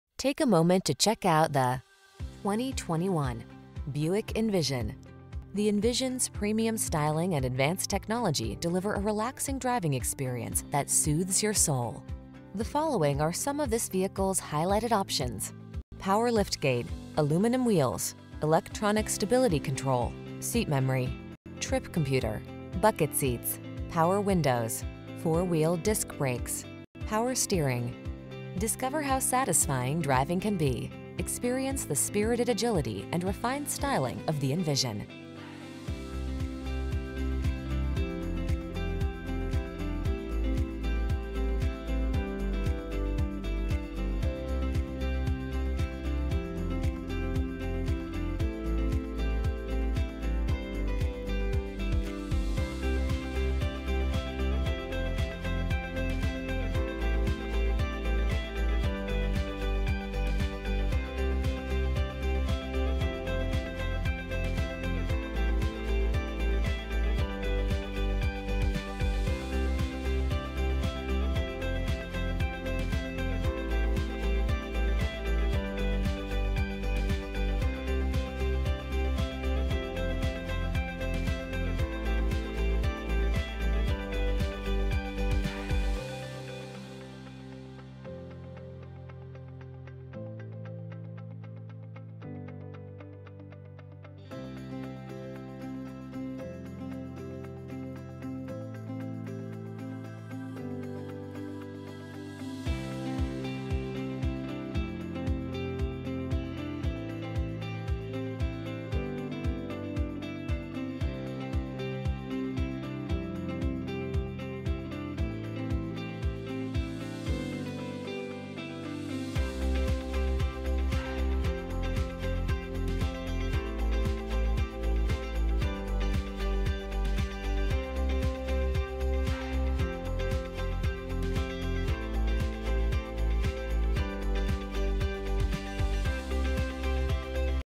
2021 Buick Envision Kenosha, Gurnee, Antioch, Burlington, Racine, WI G21363
Dark Moon Blue Metallic New 2021 Buick Envision available in Kenosha, Wisconsin at Palmen Buick GMC Cadillac. Servicing the Gurnee, Antioch, Burlington, Racine, WI area.

2021 Buick Envision Dark Moon Blue Metallic Essence 4D Sport UtilitybrAWD 9-Speed Automatic 2.0L TurbochargedbrbrbrRecent Arrival! 22/29 City/Highway MPG